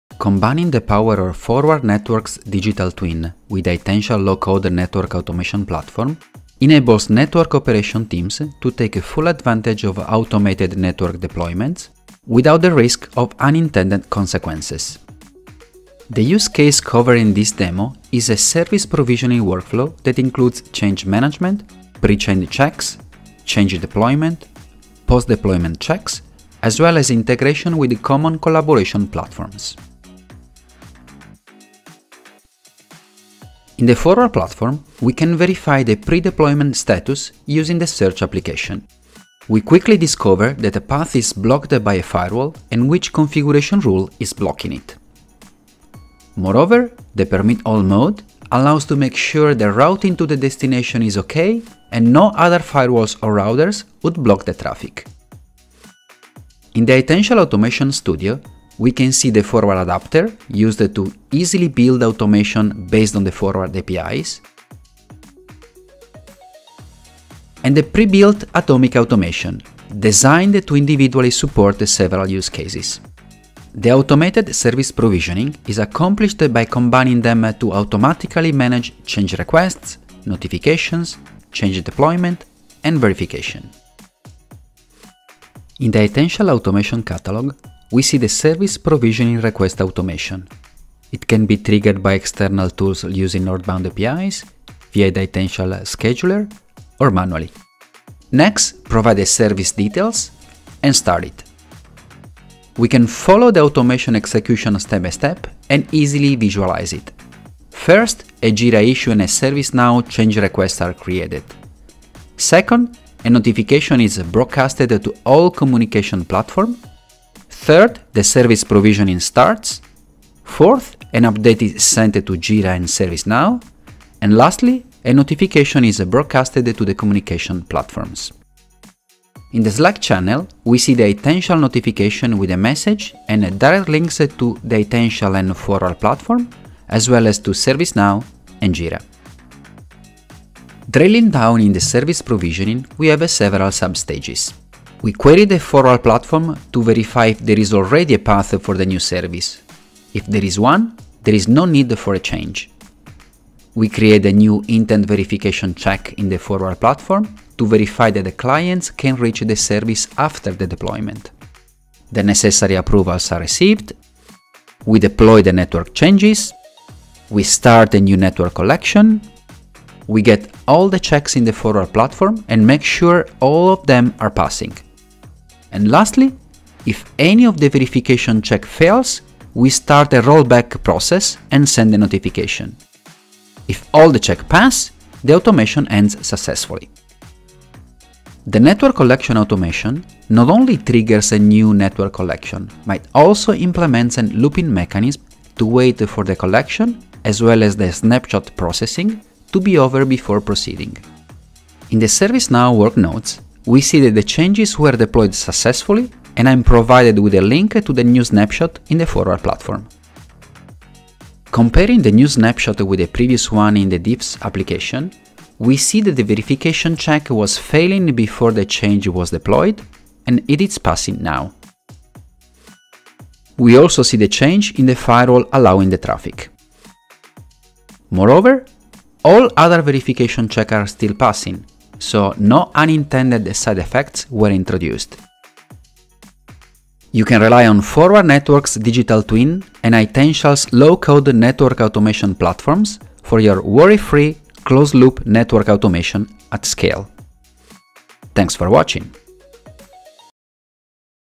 How to Achieve Closed Loop Network Automation with Itential + Forward Networks | ONUG
Network automation is a key component of closed-loop automation, but it is not complete without the proper safeguards provided by network verification. Itential’s Senior Solution Architect, Mike Elrom and Forward Networks Director of Technical Marketing, Fabrizio Maccioni demo the technology integrations of Itential’s Automation Platform with Forward Networks platform that assures network operators that any changes made in the network had the intended effect and that no unexpected behaviors were introduced when automating network changes.